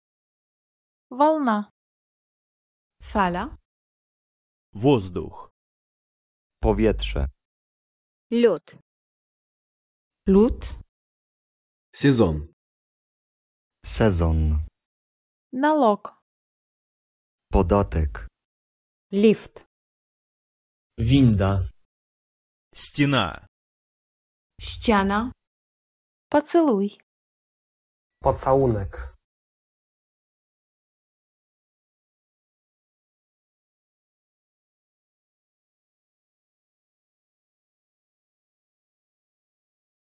#55 - Język ROSYJSKI - 500 podstawowych słów. Kurs języka rosyjskiego.
# 500 podstawowych słów języka rosyjskiego.

1  волна  fala
2  воздух  powietrze
3  лёд  lód
4  сезон  sezon
5  налог  podatek
6  лифт  winda
7  стена  ściana
8  поцелуй  pocałunek

  Niektóre z przedstawionych słów mogą mieć dodatkowe znaczenia. Wybierając tłumaczenie słowa, opieraliśmy się na najczęściej wykorzystywane znaczenie.
  Pewna część słów jest udźwiękowiona specjalnie dla naszego kanału. Udźwiękawianie niektórych słów, wzięto z bezpłatnych źródeł w Internecie, takich jak:

Język rosyjski on-line
Język rosyjski samodzielnie
Język rosyjski bezpłatnie